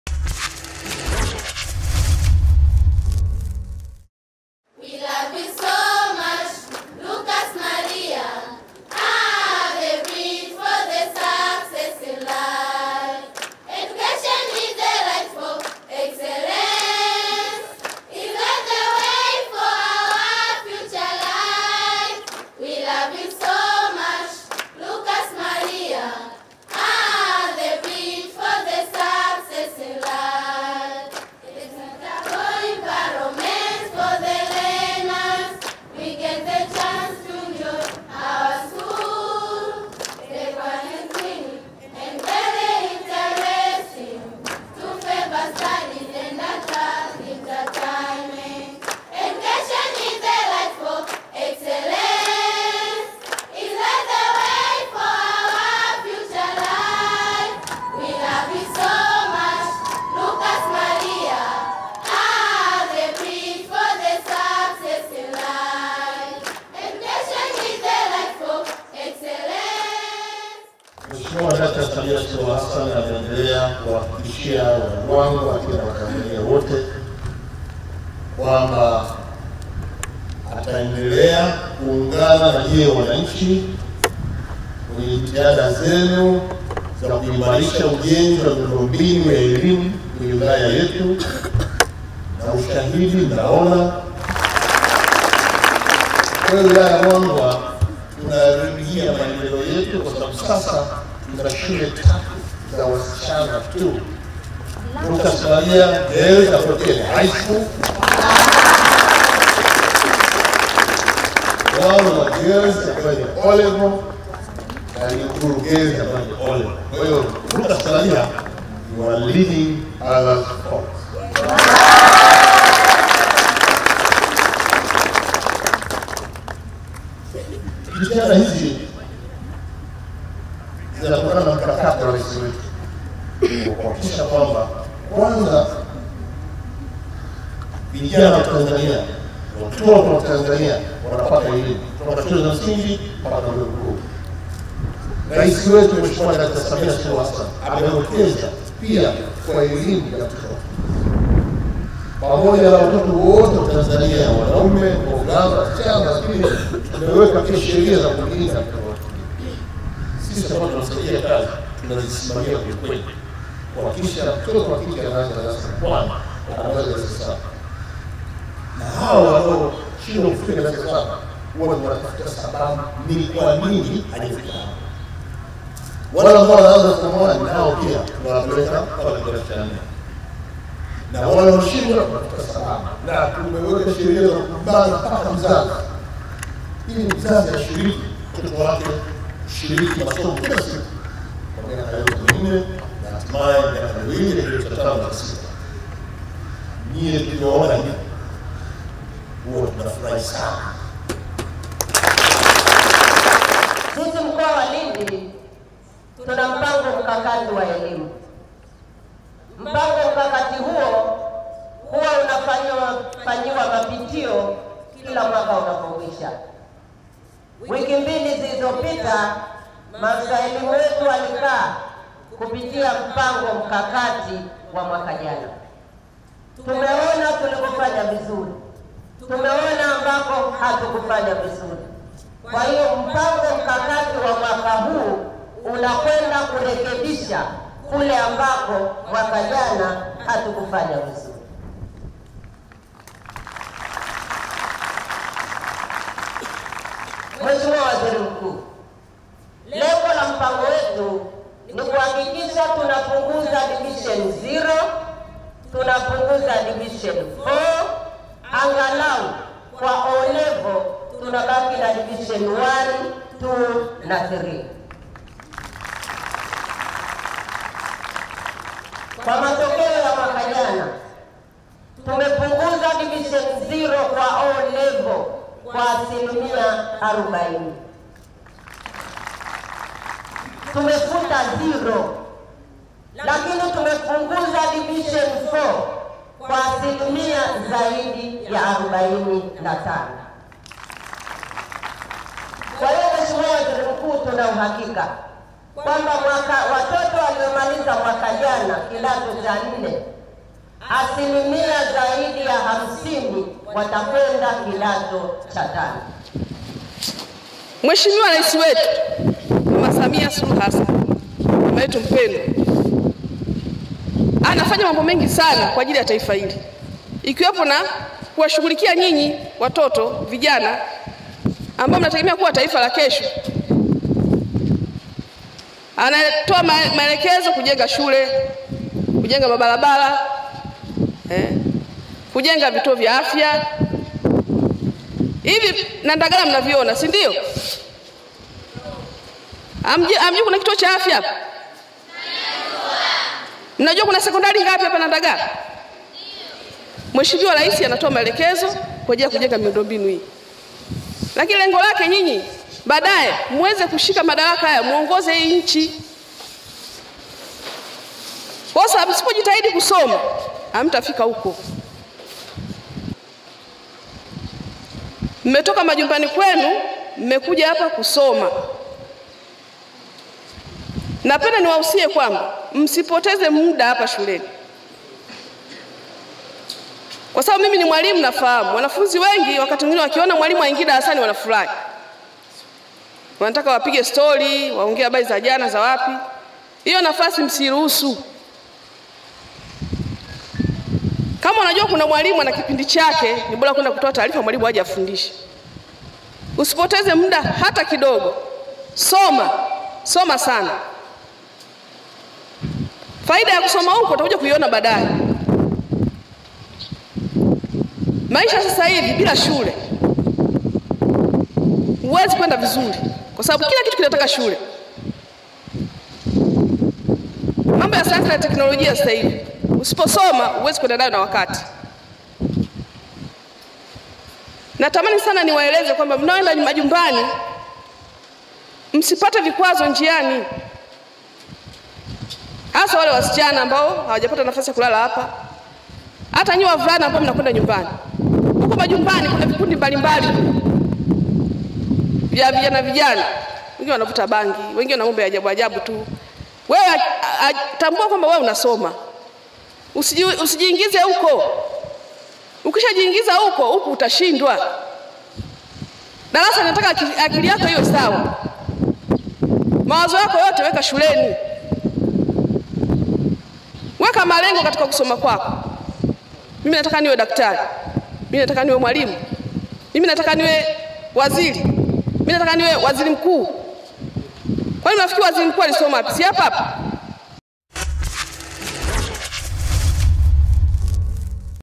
WAZIRI MKUU AMPA TANO RAIS SAMIA WAZIWAZI, WATOTO WOTE TANZANIA LAZIMA KUSOMA
Waziri wa Tanzania Kassim Majaliwa amesema serikali itaendelea kuungana na wananchi katika kuhimarisha miundombinu ya elimu ili kuhakikisha watoto wa Tanzania wanapata Elimu Bora.news update